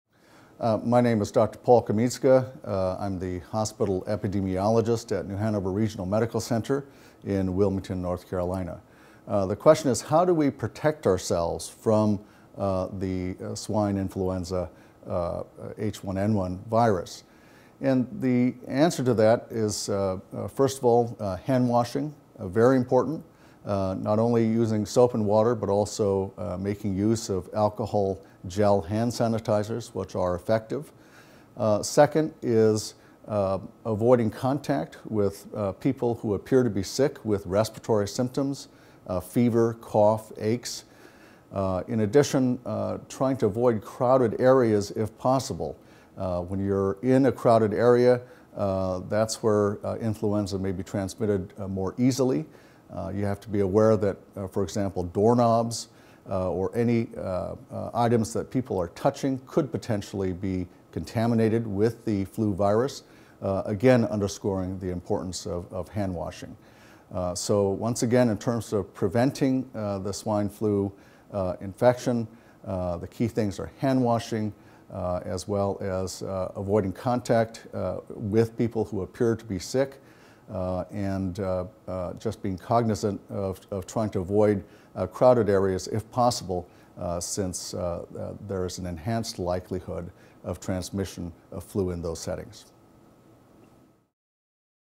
Swine Flu : How to Protect Yourself Against Swine Flu
Protection against swine flu is possible through frequent and thorough hand-washing, both with soap and with alcohol gel sanitizers, and avoiding crowded areas where people might exhibit symptoms. Take all measures to stay clean and sanitized with health information from an epidemiologist in this free video on the swine flu.

Expert: Dr. Paul Kamitsuka
Bio: Dr. Paul Kamitsuka, M.D., DTM&H, is the epidemiologist for New Hanover Regional Medical Center in Wilmington, N.C.